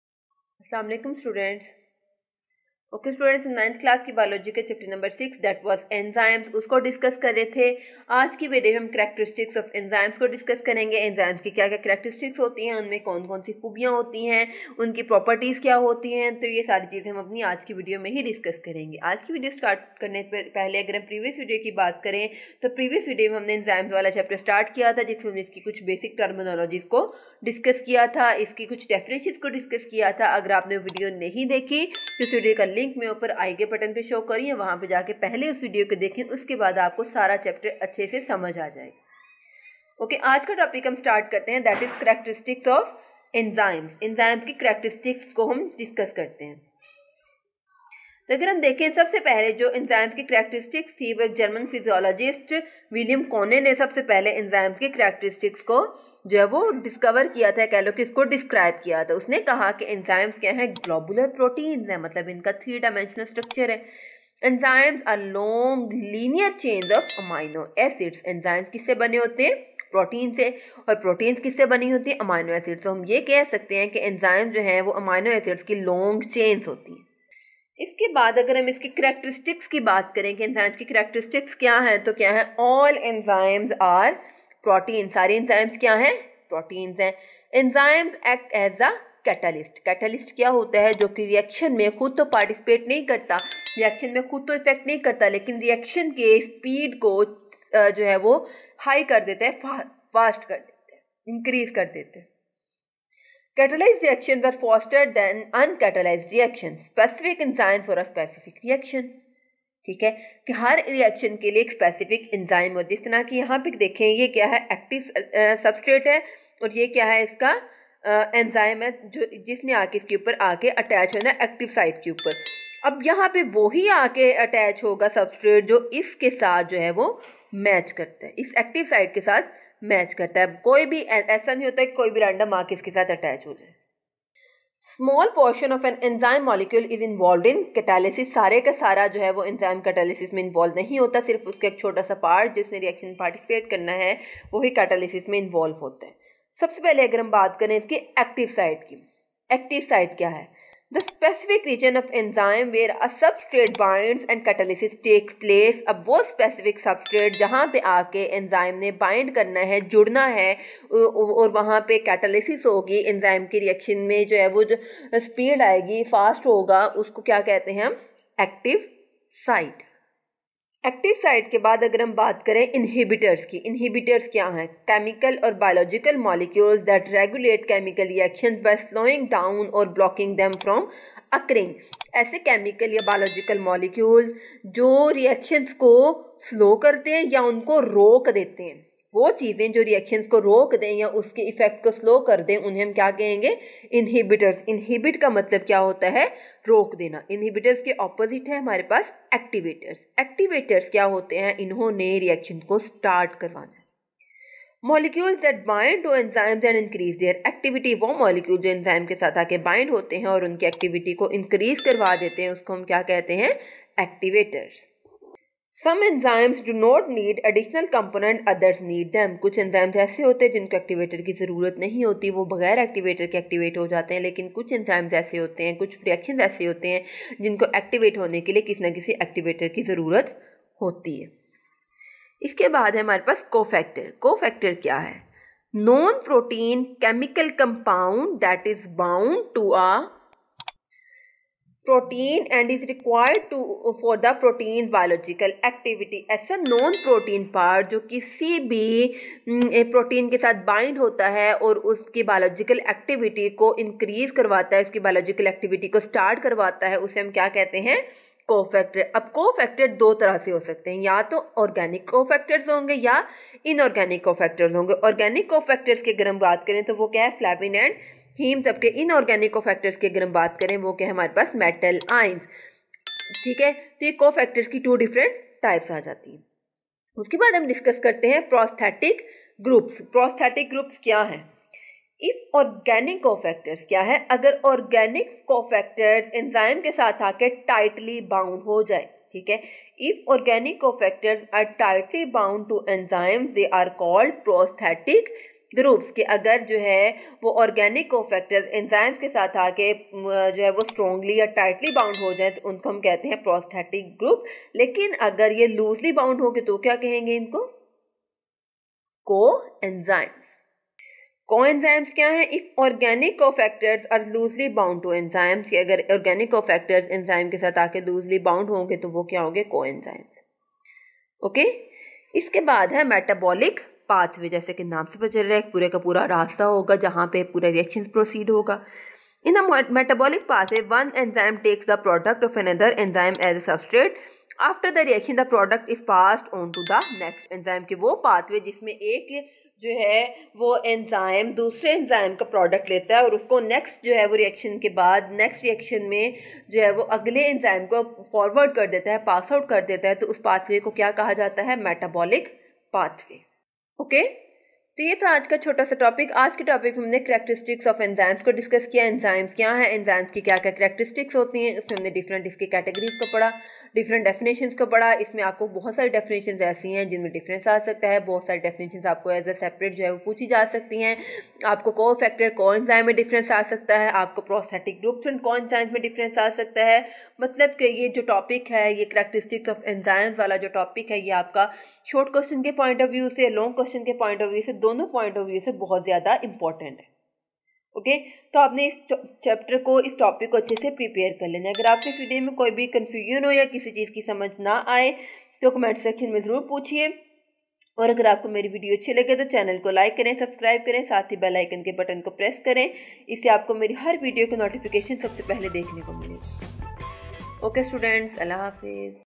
Exploring the Characteristics of Enzymes in Biology Class 9 | Enzymes in Hindi & Urdu
🔬 Dive into the fascinating world of enzymes as we explore the characteristics and functions of these biological catalysts in today's video! From understanding their properties to their crucial role in biochemical reactions, here's what we'll cover:

Discovering Enzyme Characteristics: 📚
Exploring the groundbreaking work of German physiologist Wilhelm Kühne in defining enzyme characteristics.
Unraveling the three-dimensional structure of enzymes and their composition from proteins and amino acids.

Understanding Enzyme Function: ⚙️
Delving into how enzymes facilitate biochemical reactions by lowering activation energy.
Exploring the concept of catalysis and the distinction between catalyzed and non-catalyzed reactions.

Activating Enzymes: 🚀
Learning about the activation process and how some enzymes require additional components to enhance their activity.
Discussing factors and non-protein chemical compounds essential for protein activation.

Inhibiting Enzyme Activity: 🛑
Understanding how certain molecules slow down or block biochemical reactions, affecting enzyme activity.
Exploring the significance of inhibitors and their impact on biological processes.

Factor X: Unveiling Non-Protein Chemical Compounds: 💡
Examining the role of non-protein compounds bound to proteins and their influence on biological activity.

Unraveling how these compounds contribute to enhancing protein function and biochemical activity.
Join us as we unravel the mysteries of enzymes and gain a deeper understanding of their pivotal role in biological processes! 🌟